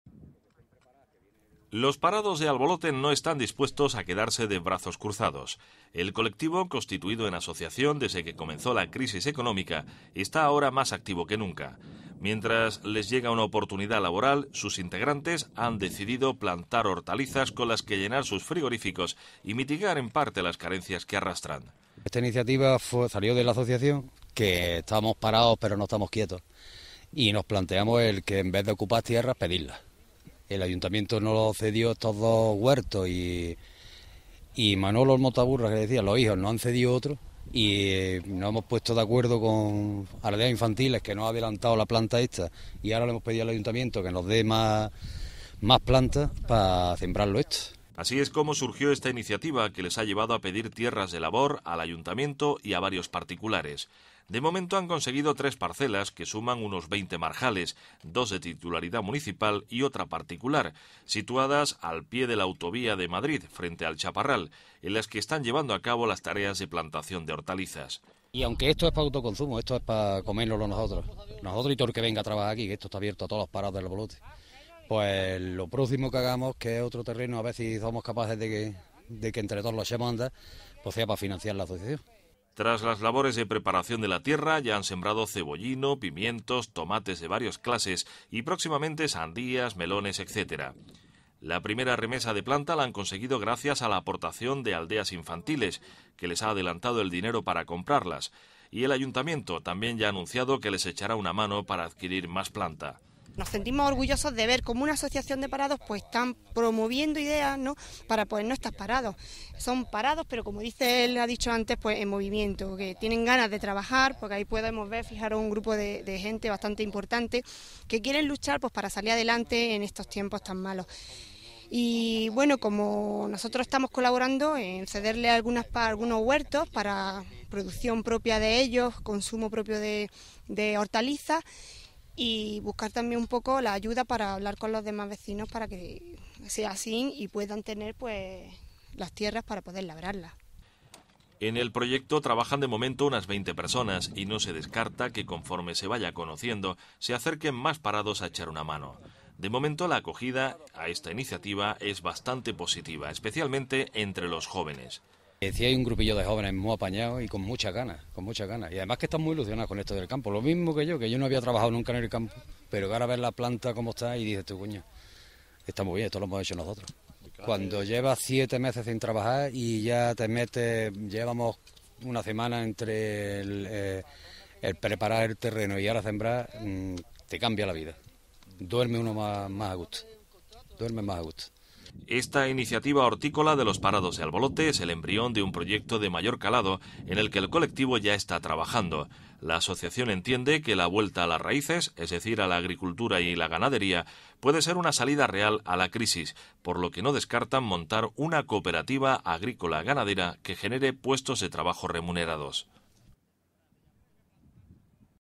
PARADOS DE ALBOLOTE CULTIVAN LA TIERRA PARA AUTOCONSUMO
La asociación de parados de Albolote ha conseguido tres parcelas, dos de titularidad municipal y otra particular, en las que están plantando hortalizas, de momento solo para autoconsumo.